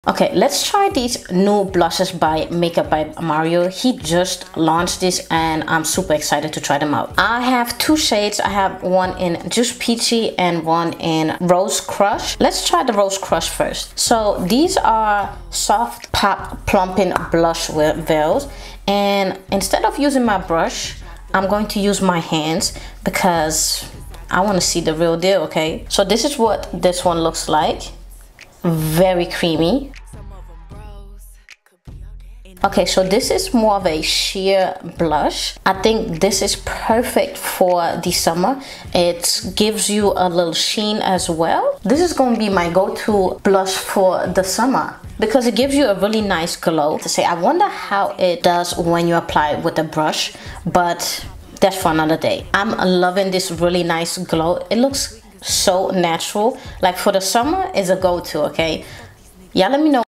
NEW Makeup By Mario VEIL BLUSHES 🤌🏾
Love these beautiful blushes ! They give me a nice glow very creamy! Perfect for the summer .